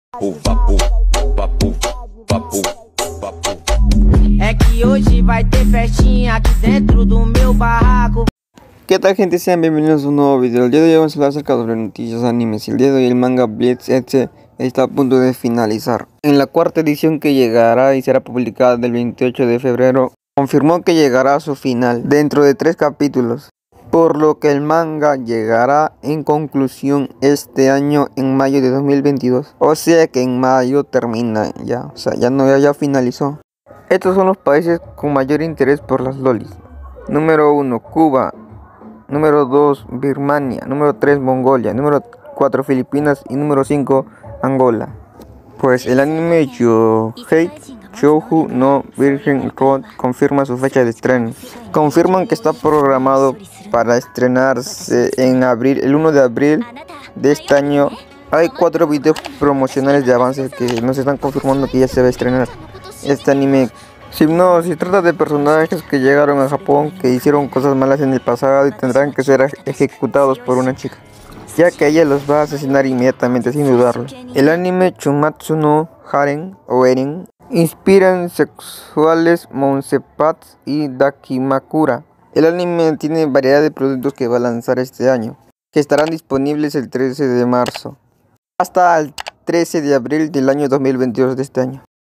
Shokei Shoujo no Virgin Road confirma su fecha de estreno
En el sitio oficial para la adaptación al anime de las novelas ligeras escritas por Mato Satou e ilustradas por Nilitsu, Shokei Shoujo no Virgin Road (The Executioner and Her Way of Life), se publicaron cuatro videos promocionales protagonizados por distintos personajes del proyecto. Los videos de Menou, Akari, Momo y Ashuna confirman que el estreno está programado para el próximo 1 de abril en Japón